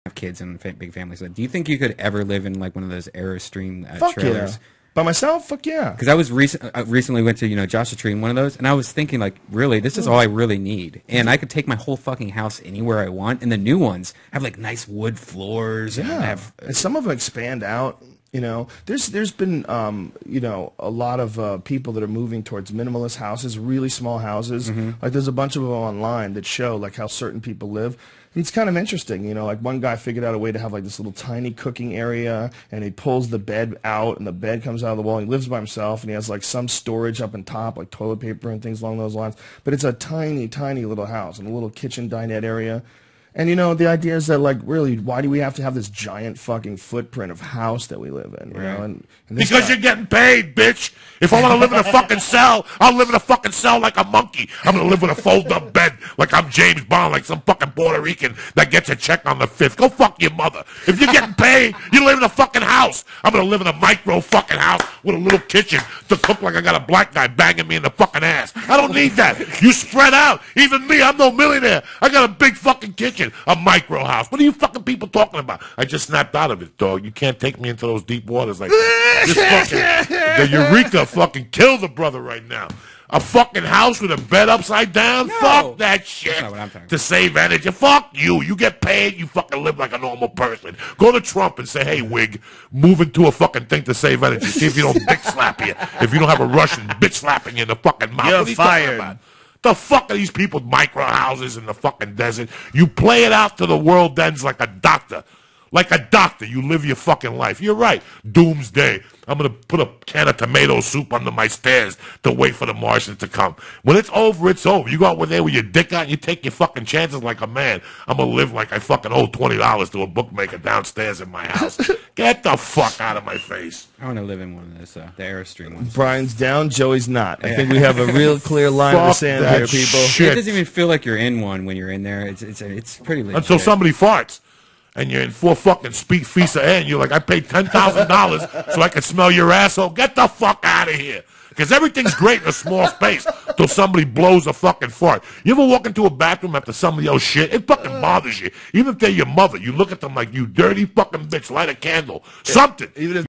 Joey Diaz BEST RANT EVER!
Joey is completely blitzkrieg'd when Joe starts talking about minimalist housing and BOOM he explodes to life and shits on the whole idea! (I realize this isn't Joey's best rant but it definitely stacks up to his other classics)